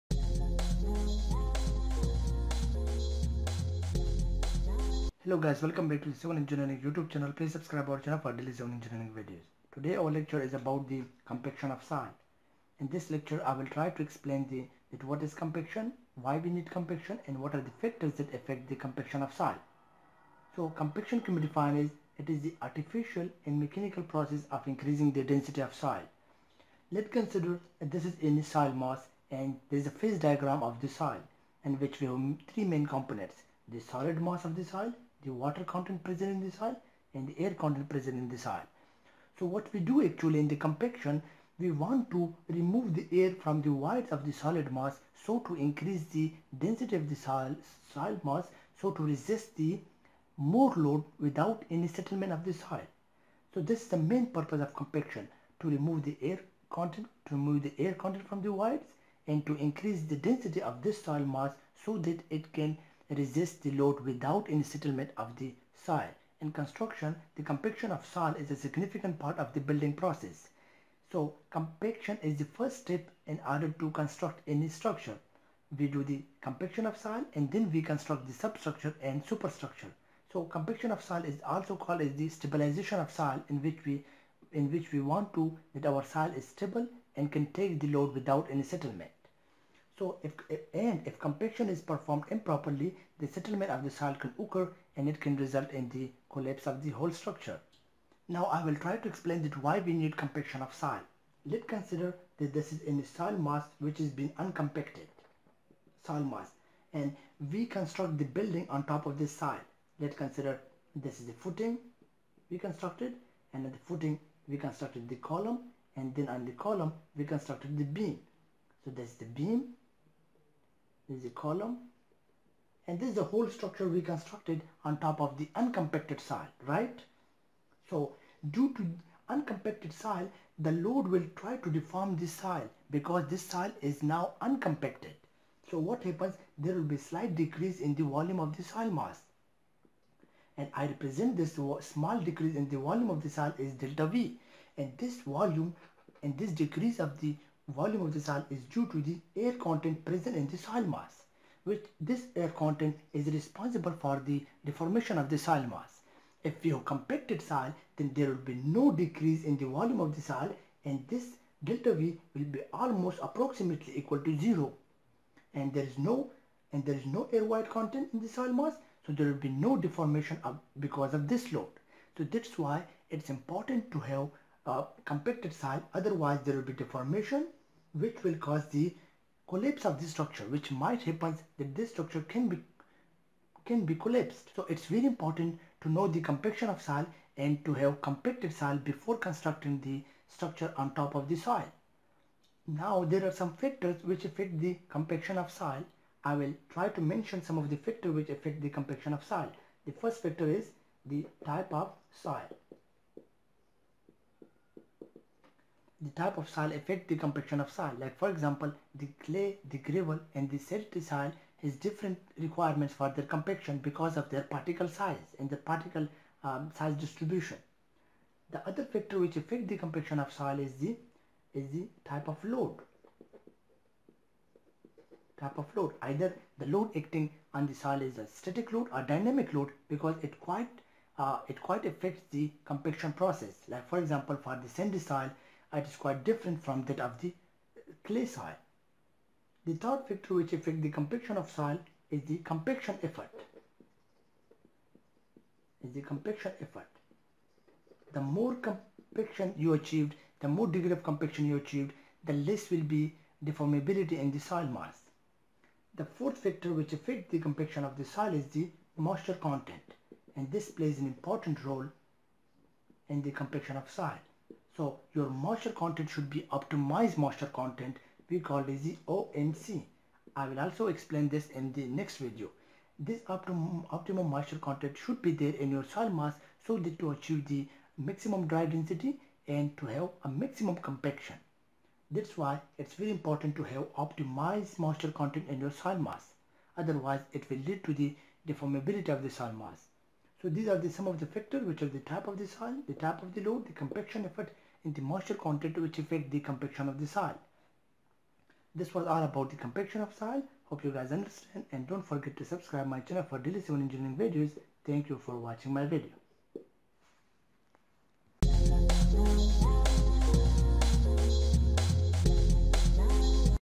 Compaction of Soil || Factors affecting compaction of soil
This video  is about the compaction of soil, Compaction of soil in soil mechanics can be defined as " The artificial and mechanical process of increasing the density of soil". There are various factors which affect the compaction of soil;
1. Type of soil
2. Type of load
3. Compaction effort
4. Moisture content